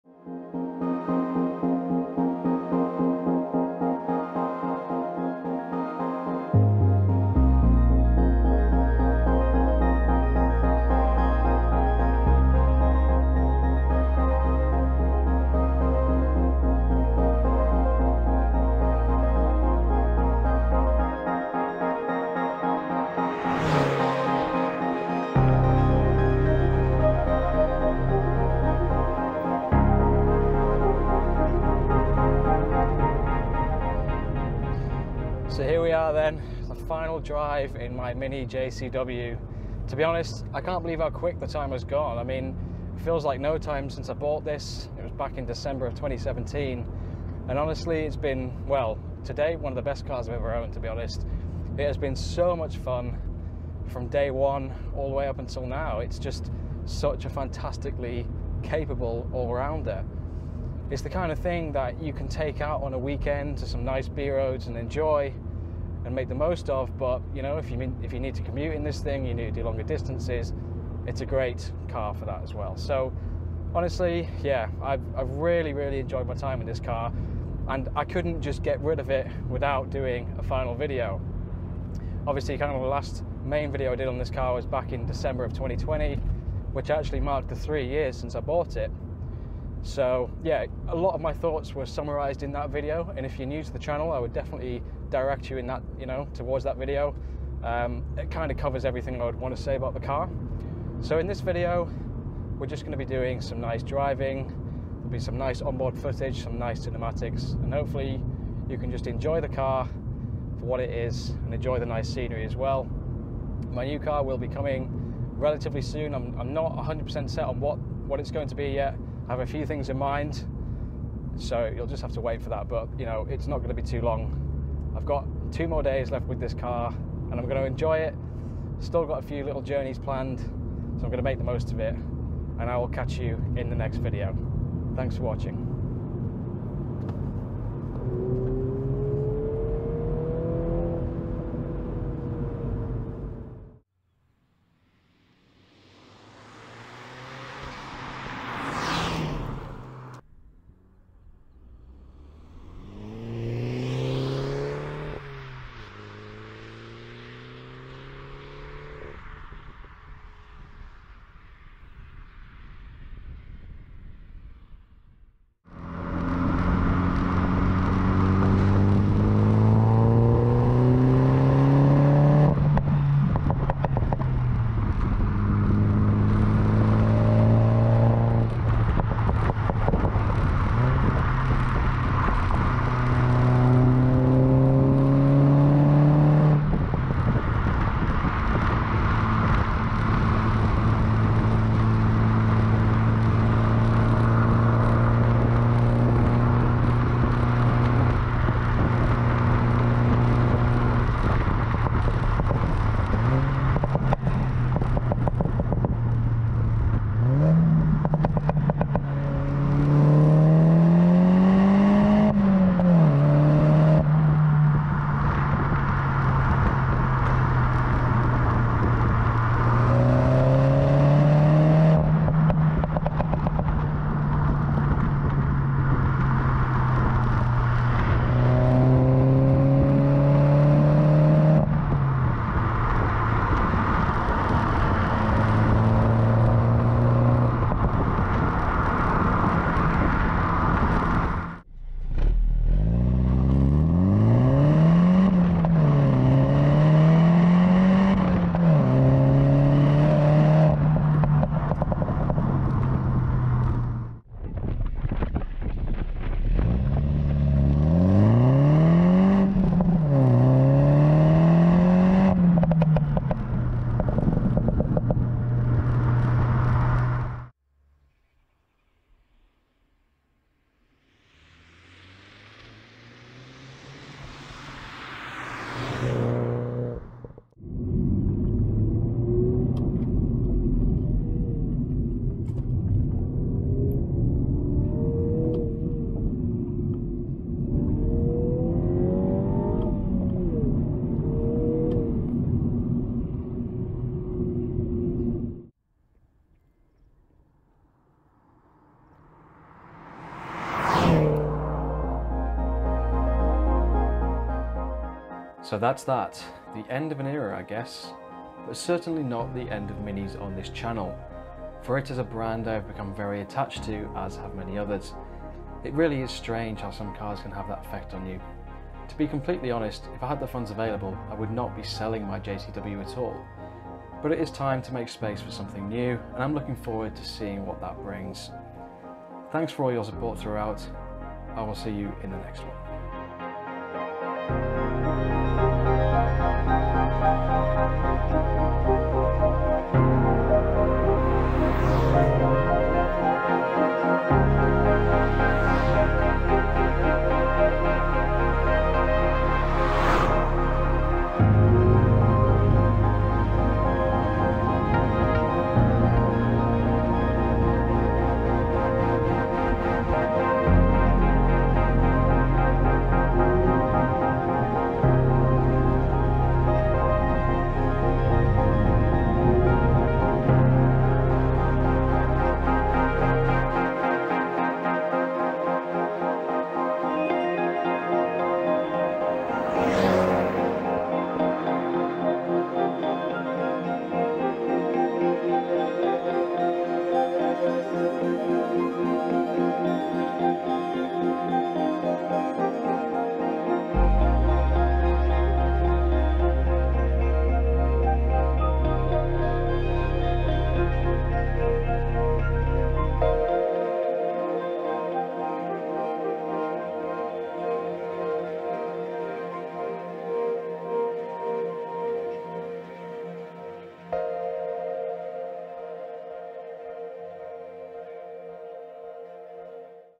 Final Drive In My MINI JCW (F56) | End of an era... for now
I am truly sad to see the JCW go, but it's time to make space for something new and I'm excited about what that will bring.

Thanks for all the support throughout my period of ownership, and rest assured that this is not the end of MINI content on the channel!

Check us out on Instagram for more of our content!